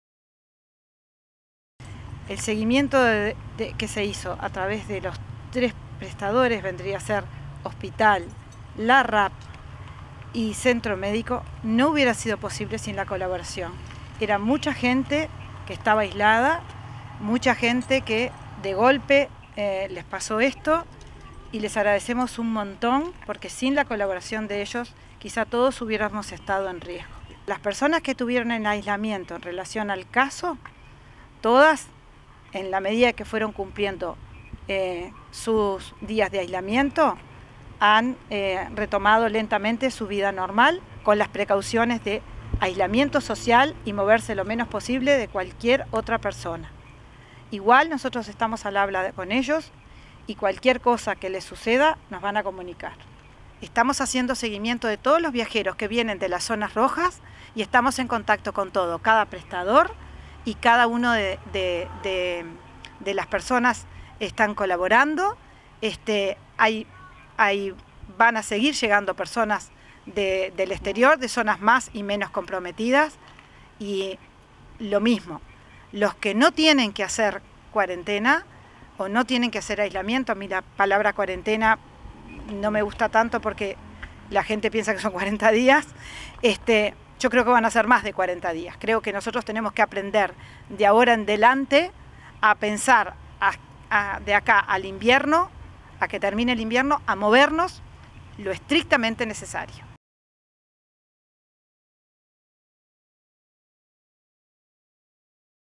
Importante seguimiento ante los casos de aislamiento por parte del HRS, RAP y CAM.
A sido importante el seguimiento ante los casos de aislamiento por parte del HRS, RAP y CAM. Se continua haciendo el seguimiento a los viajeros que llegan de zonas rojas resaltando que serán más de 40 días, habrá que moverse lo estrictamente necesario.